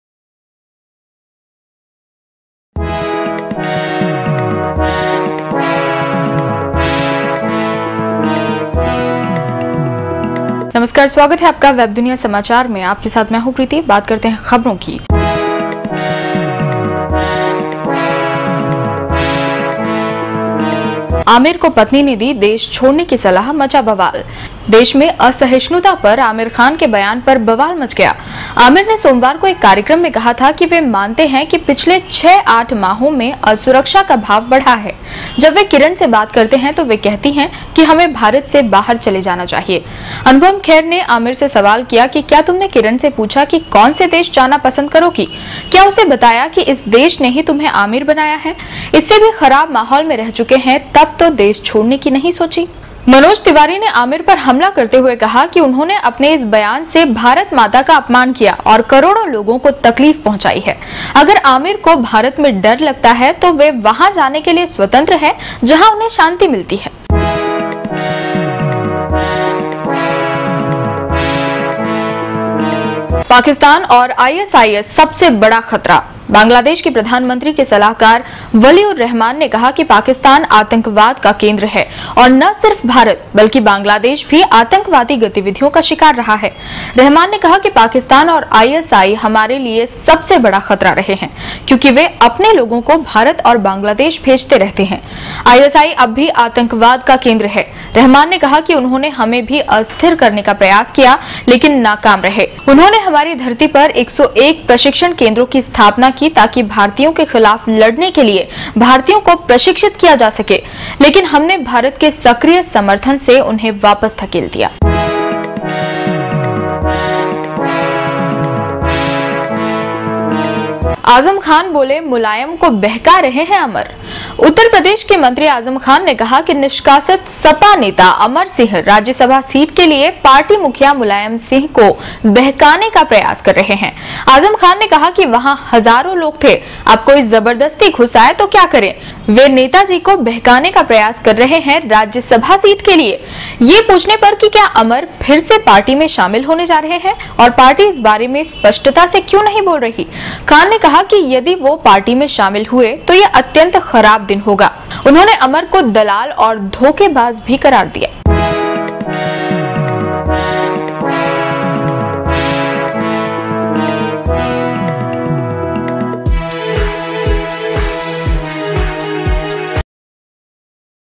आमिर को पत्नी ने दी देश छोड़ने की सलाह | Aamir's wife Kiran wants to leave India
देश में असहिष्णुता पर आमिर खान के बयान पर बवाल मच गया। आमिर ने सोमवार को एक कार्यक्रम में कहा था कि वह मानते हैं कि पिछले छह-आठ माहों में असुरक्षा का भाव बढ़ा है। जब वह किरण से बात करते हैं तो वह कहती हैं कि हमें भारत से बहार चले जाना चाहिए। अनुपम खेर ने आमिर से सवाल किया कि क्या तुमने किरण से पूछा कि कौन से देश जाना पसंद करोगी? क्या उसे बताया कि इस देश ने ही तुम्हे आमिर बनाया है? इससे भी खराब माहौल में रह चुके है तब तो देश छोड़ने की नहीं सोची।